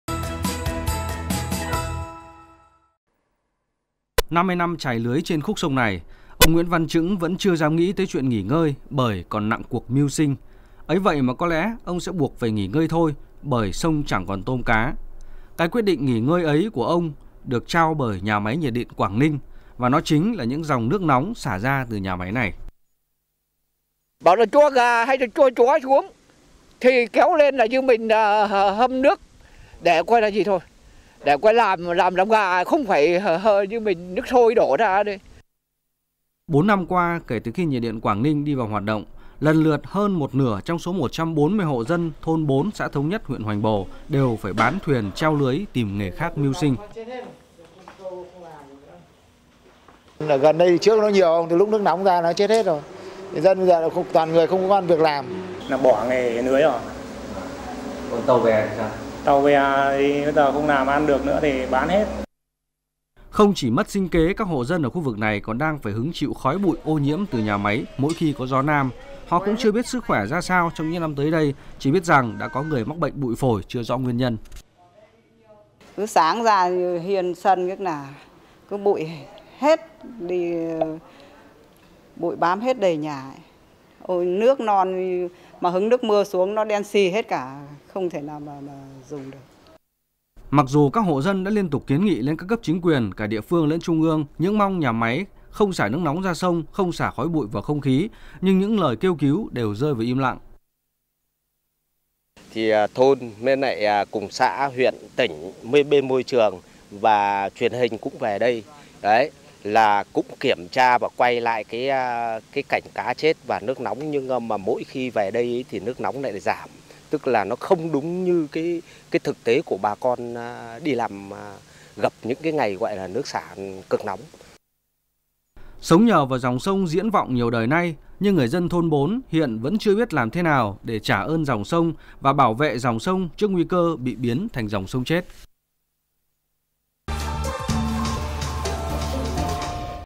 Nhà máy nhiệt điện xả nước ‘mát’, dân kêu trời | VTC
VTC | Hàng trăm hộ dân làm nghề chài lưới trên sông Diễn Vọng (Hoành Bồ, Quảng Ninh) bị mất kế sinh nhai khi nhà máy Nhiệt điện Quảng Ninh thường xuyên xả nước nóng ra sông khiến tôm cá chết nhiều.